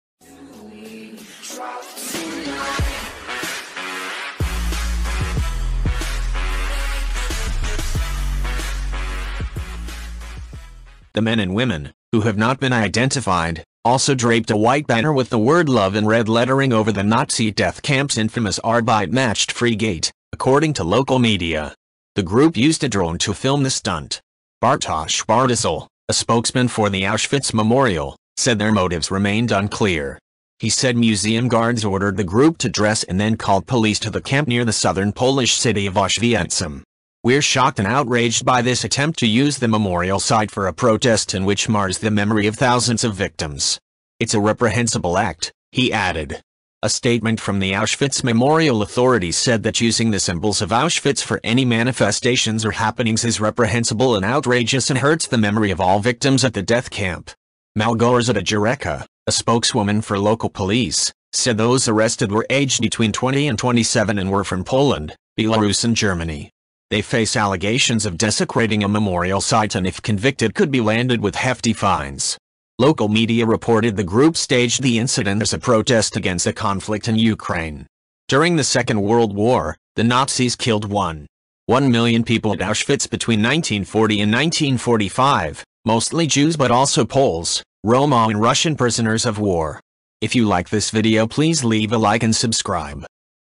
Naked protesters arrested after killing sheep at Auschwitz death camp
A group of 11 people have been detained after stripping naked, slaughtering a sheep and chaining themselves together at the entrance to Auschwitz.

The men and women, who have not been identified, also draped a white banner with the word "love" in red lettering over the Nazi death camp's infamous Arbeit macht frei gate, according to local media.

The group used a drone to film the stunt.

Bartosz Bartyzel, a spokesman for the Auschwitz memorial, said their motives remained unclear.

He said museum guards ordered the group to dress and then called police to the camp near the southern Polish city of Oswiecim.

"We're shocked and outraged by this attempt to use the memorial site for a protest and which mars the memory of thousands of victims. It's a reprehensible act," he added.

A statement from the Auschwitz memorial authorities said that "using the symbols of Auschwitz for any manifestations or happenings is reprehensible and outrageous" and "hurts the memory of all victims" at the death camp.

Malgorzata Jurecka, a spokeswoman for local police, said those arrested were aged between 20 and 27 and were from Poland, Belarus and Germany.

They face allegations of desecrating a memorial site and if convicted could be landed with hefty fines.

Local media reported the group staged the incident as a protest against the conflict in Ukraine.

During the Second World War, the Nazis killed 1.1 million people at Auschwitz between 1940 and 1945, mostly Jews but also Poles, Roma and Russian prisoners of war.